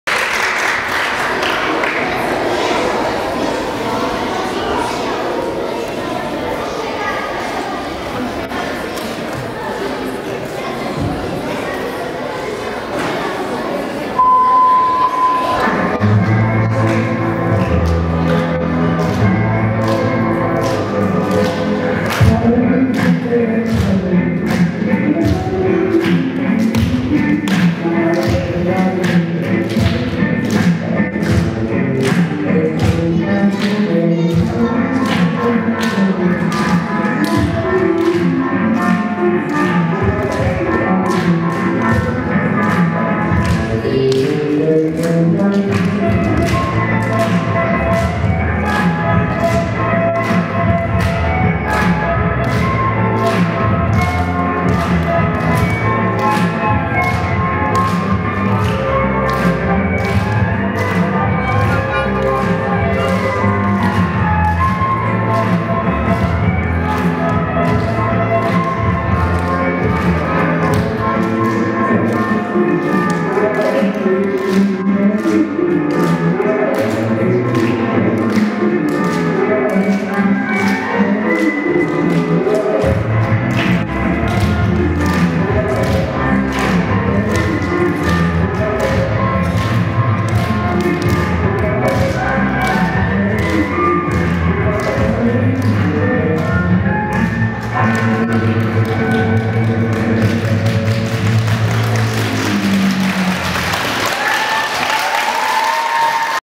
Показательные выступления с мячом. Художественная гимнастика
Выступление средней группы - номер с мячом.
В перерывах между соревнованием по художественной гимнастике в бассейне Парус среди малышей (6-7 лет)